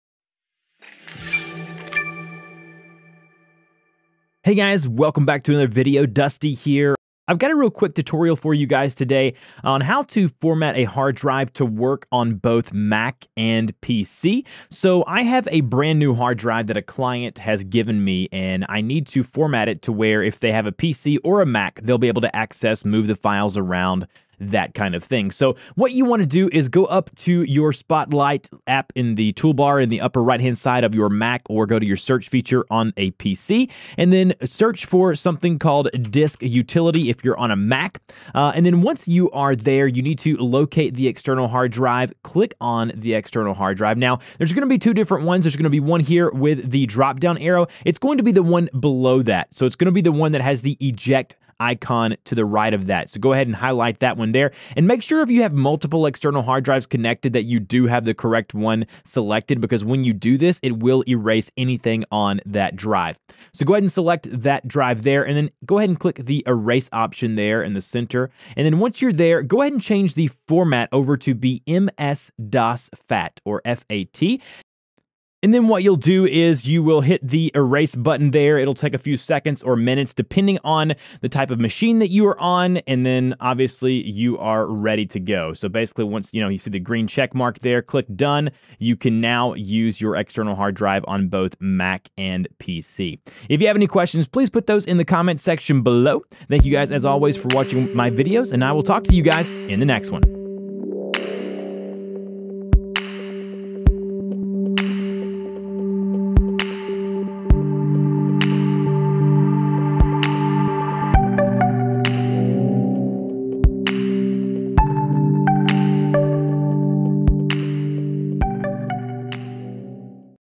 How To Format A Hard Drive To Work On Mac and PC
In this video tutorial, I show you how to easily format an external hard drive to work on both a Mac and a PC.

If you run into the issue of not having enough space format it to be the EX FAT as opposed to the one it is currently!

MY SECRET WEAPON AND BEST TOOL TO GROW A YOUTUBE CHANNEL FAST TUBEBUDDY

dmporter74